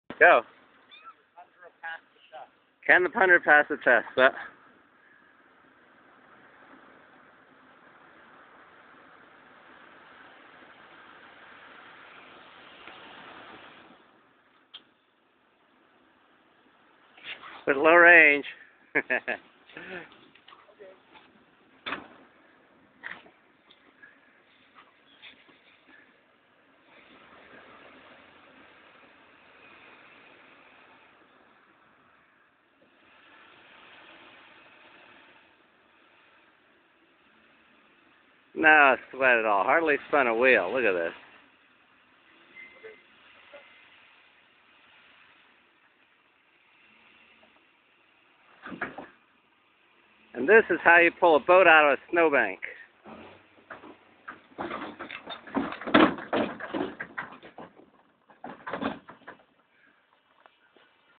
Fly Fishing with Alan
Pulling the boat out of the Bow River after flyfishing, over a snow bank.